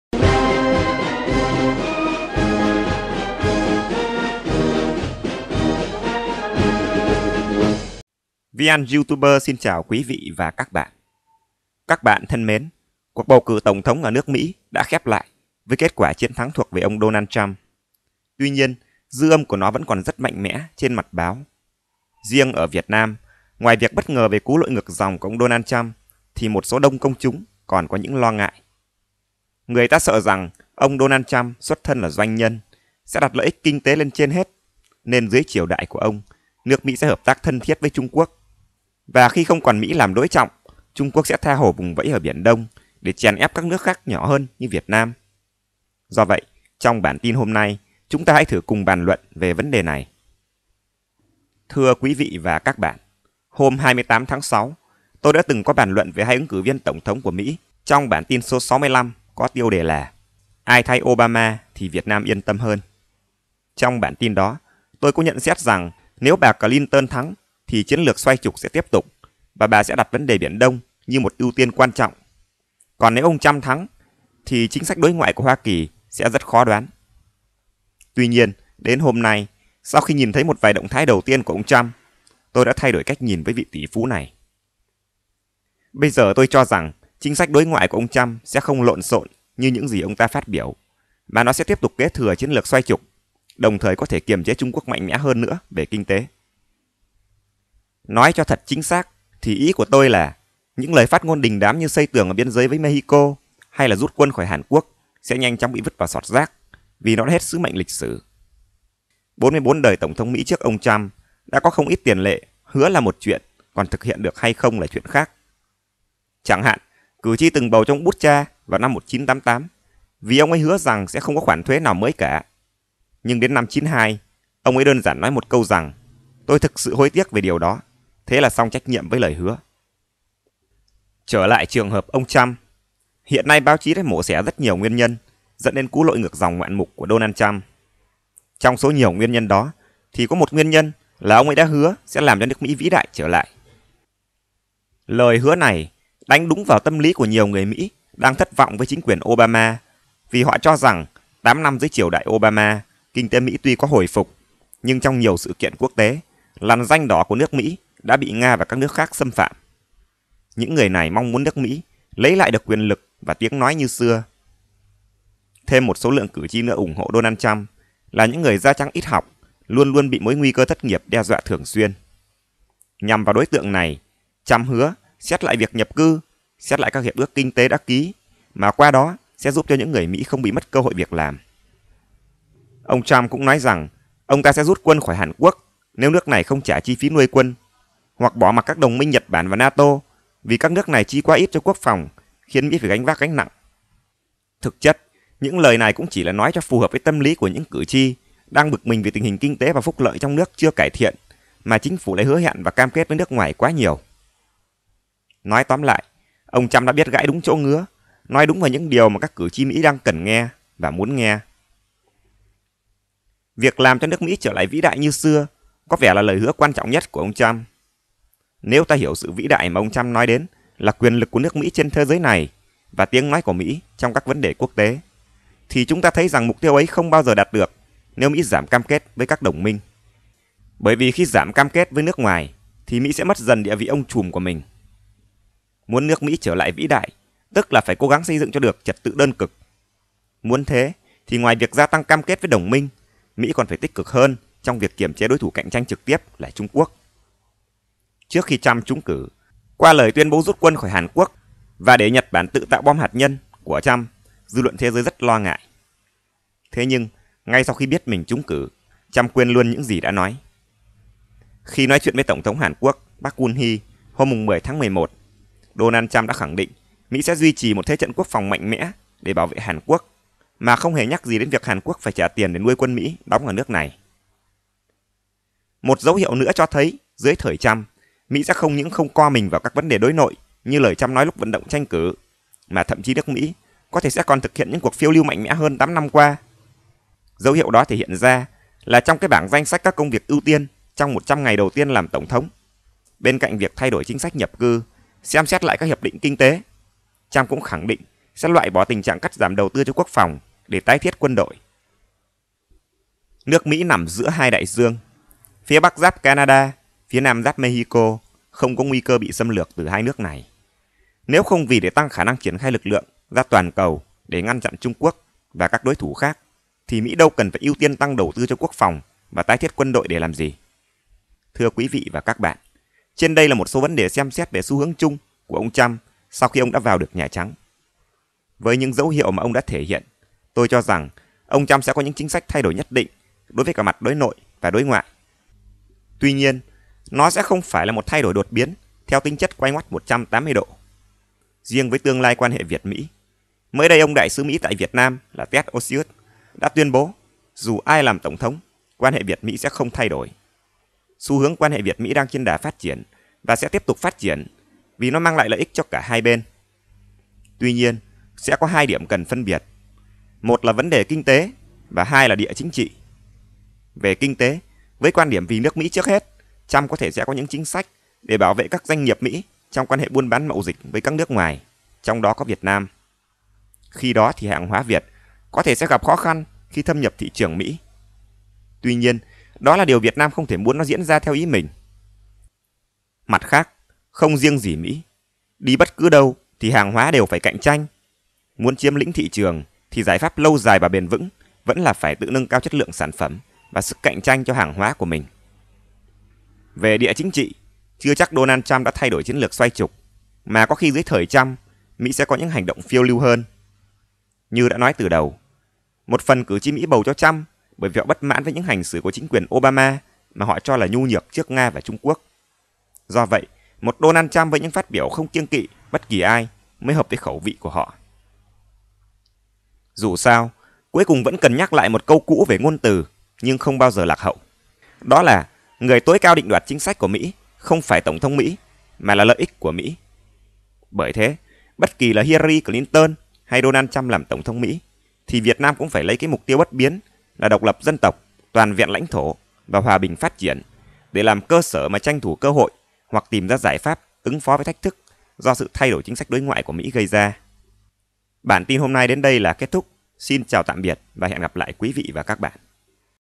Ông Trump đắc cử và lối đi nào cho VN? (181)
VN sẽ phải làm gì khi ông Trump đắc cử Tổng thống Mỹ.